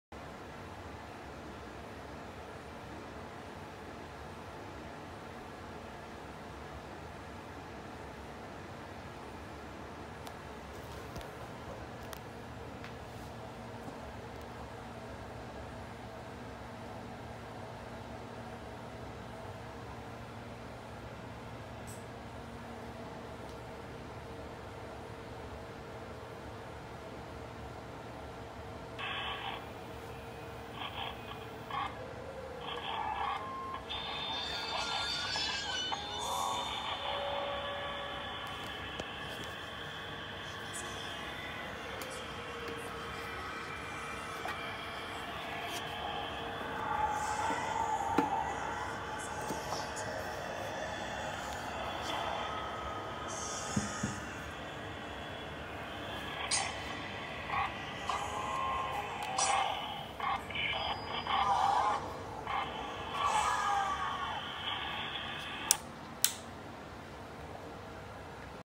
Halloween project fake CRT scare prop! AtmosFX
This is where I used the square cut out made in a previous video. It’s to replace the original monitor tube screen and make it look more realistic. Then used an LCD screen on the inside. Happy how it turned out but it is a private build not for sale. Still check us out at vcdnow.com and we can maybe do a custom order.